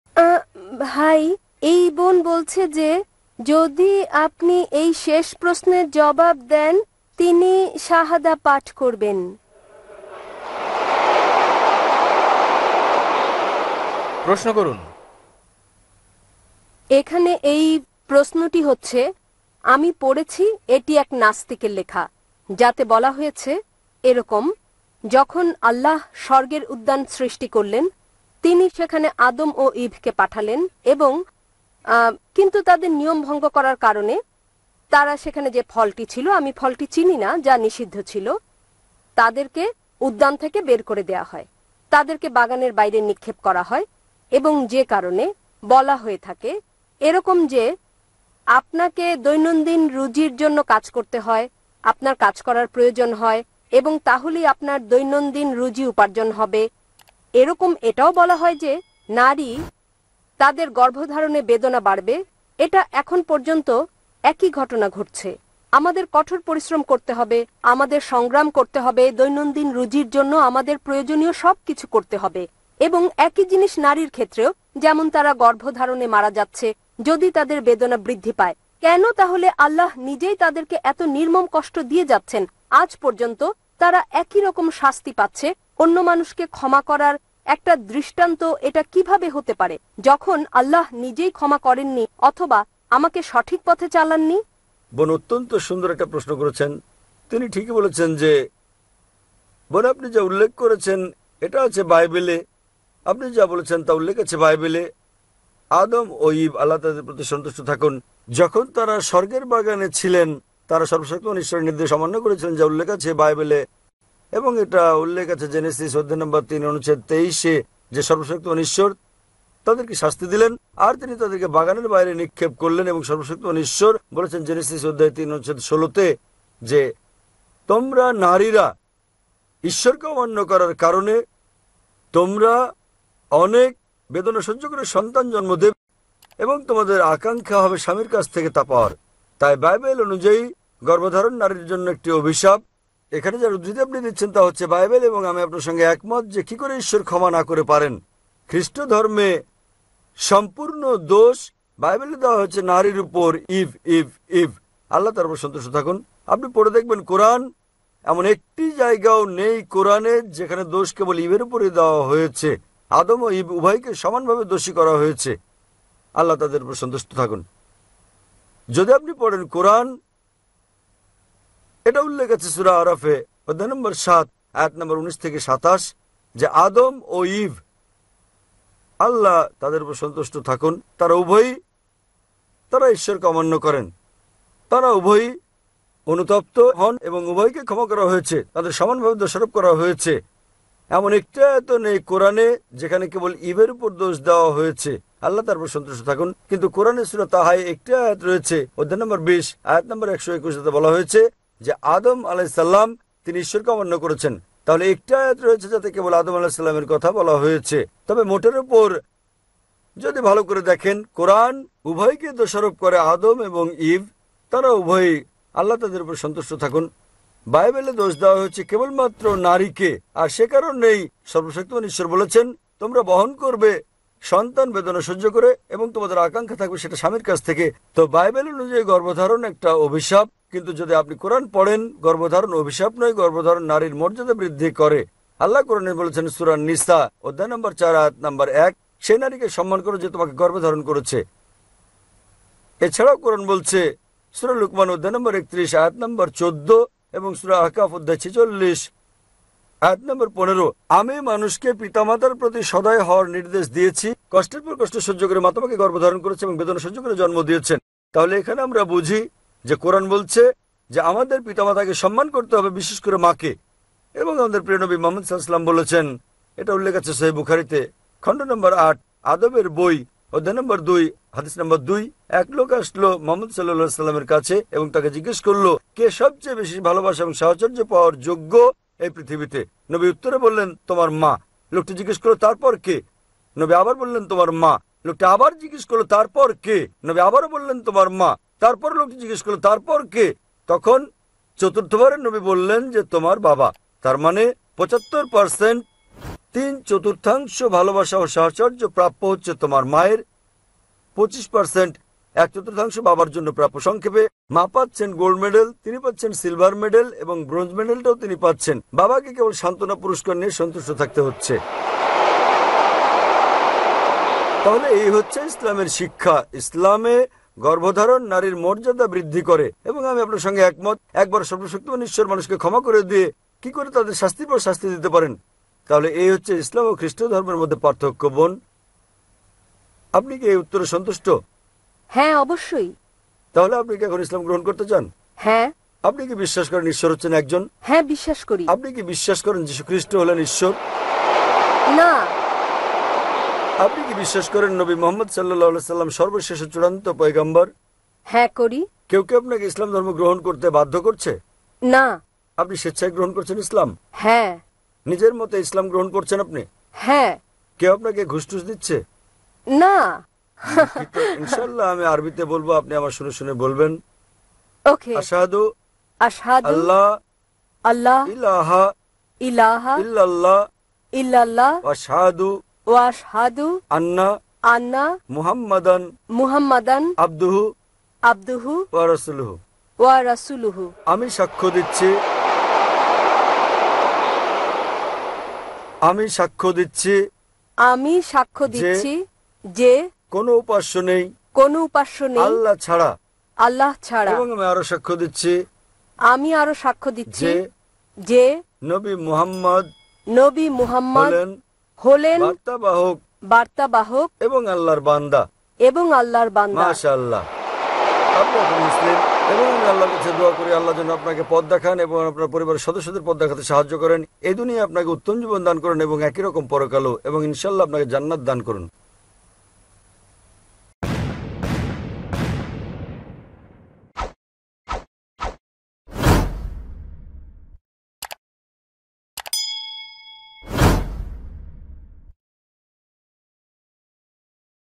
ইলসাল গ্রহন করবো যদি এই প্রশ্নের উত্তর দেন| Zakir Naik Lecture in Bangla HD| Bangla waz|was
একজন অমুসলিম তার ছয়টি প্রশ্ন ছিল। শেষ প্রশ্নের জবাব দিতে পারলে ইসলাম গ্রহন করবো। জাকির নায়েক এর উত্তরের পর সে ইসলাম গ্রহণ করে।

of Quran. Non-profit, educational or personal use tips the

আপনাদের ভালবাসা ও দোয়ায় চ্যানেলটি অনেক দূর যেতে চায়। আর লাইক কমেন্ট শেয়ার করে সাথে থাকুন।